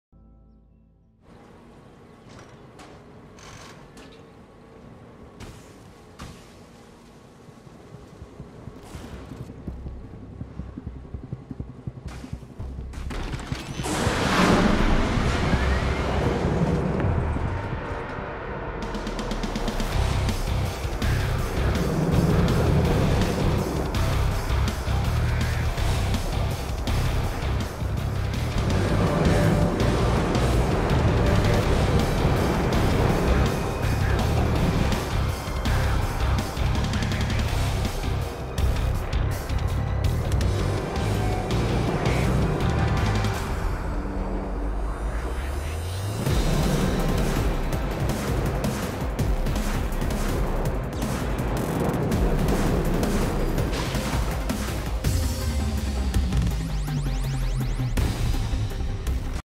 Racer X-Treme
Class: Launched - Hydraulic
Type: Sprint 500
Length: 811 m
This high-speed coaster takes you up and down and around. Themed around the formula 1 races.